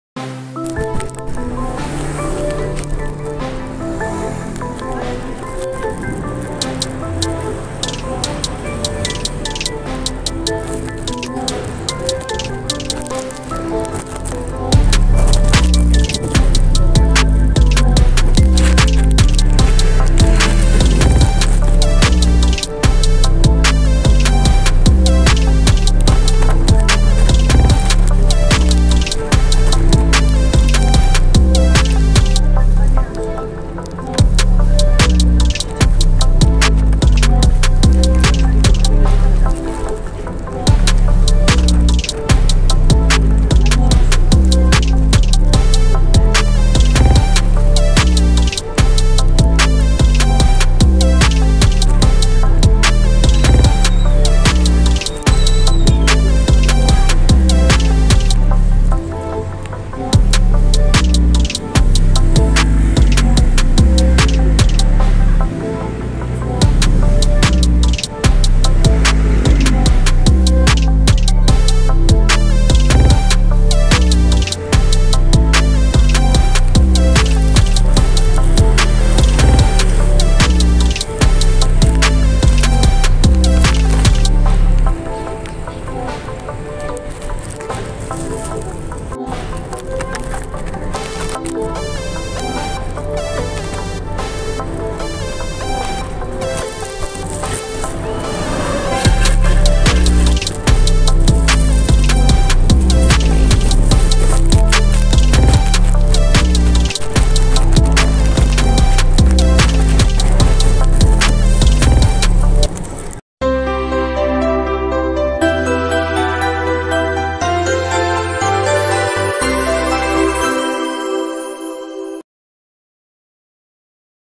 GIRL SLEEPING AT THE BUS IN THE SHOULDER OF STRANGER.real scene..no prank.
ICOVER NA LANG NATIN ANG MUKHA NYA PARA HINDI NAMAN SIYA MAPAHIYA.
FOR RESPECT NA LANG PARA SA KANYA
AT TALAGANG PUYAT SIYA DAHIL SA TRABAHO.